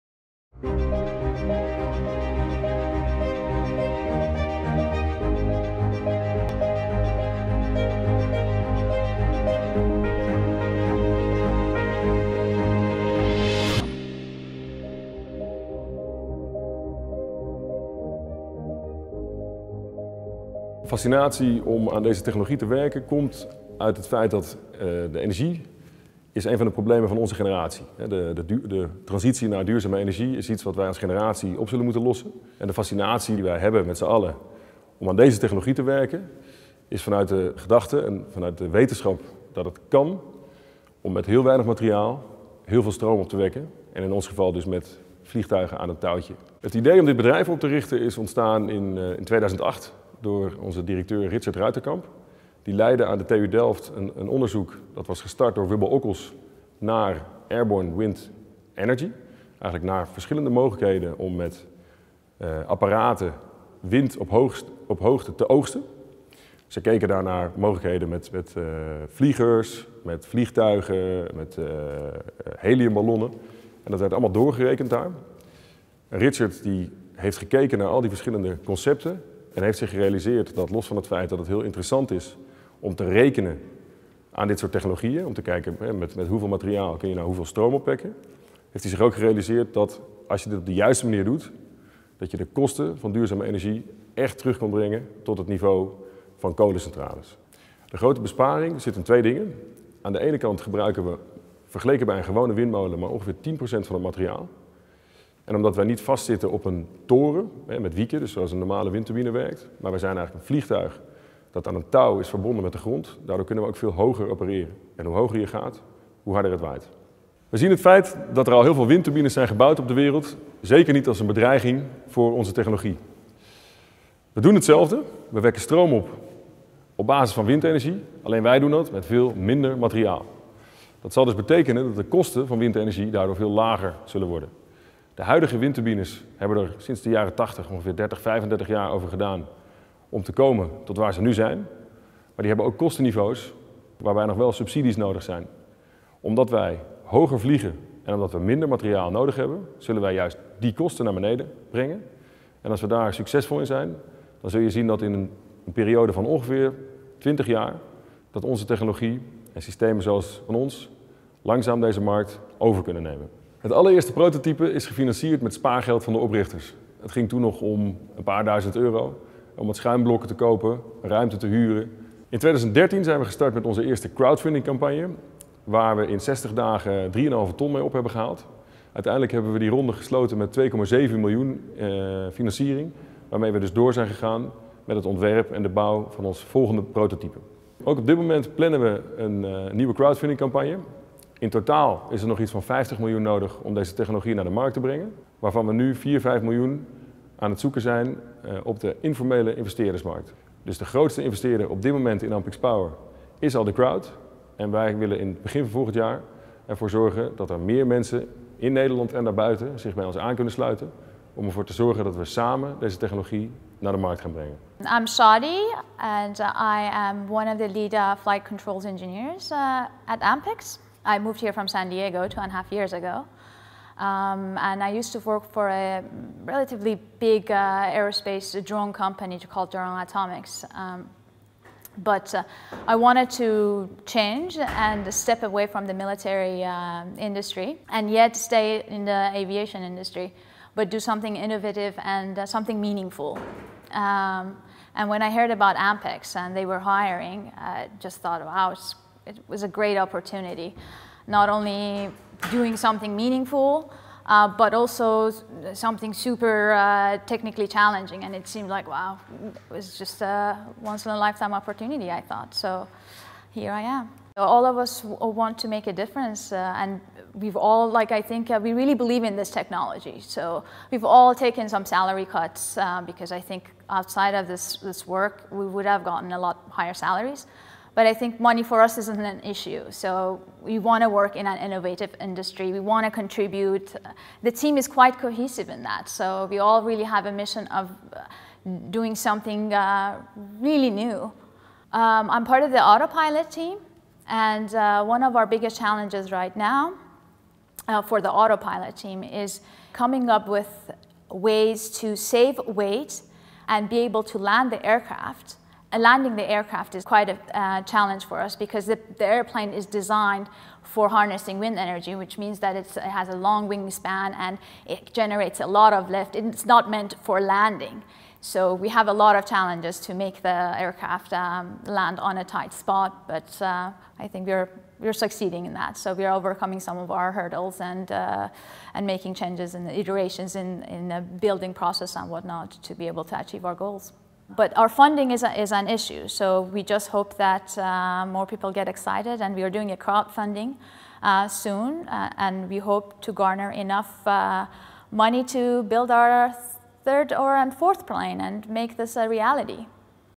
Nieuwe Energie: Ampyx Power
Wie slechts naar windmolens kijkt voor windenergie, kijkt niet ver genoeg. Ver boven de toppen van de traditionele molen kun je ook energie oogsten. Met drones. Net zo groen, bijna onzichtbaar en veel goedkoper, beloven de vliegtuigbouwers van Ampyx Power.